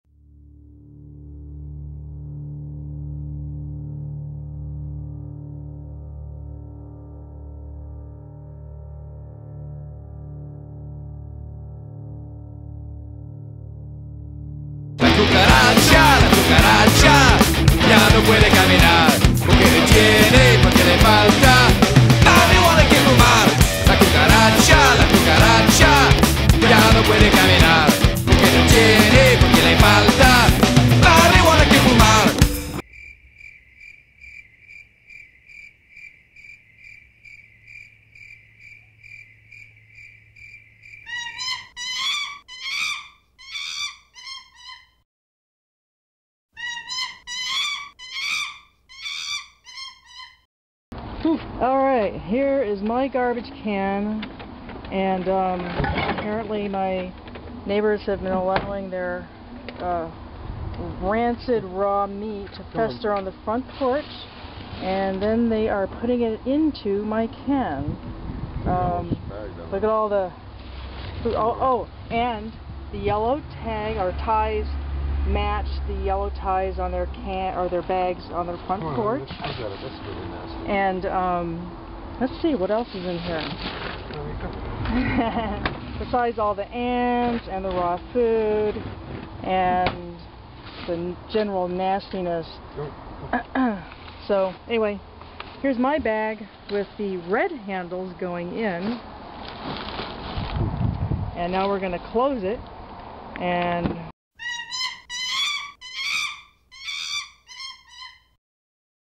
My garbage Problem
My neighbors are putting their nasty garbage in my can.  I've lodged a complaint with the City of San Rafael, Ca, but the inspector is apparently unfamiliar with local ordinances.  He didn't see a problem.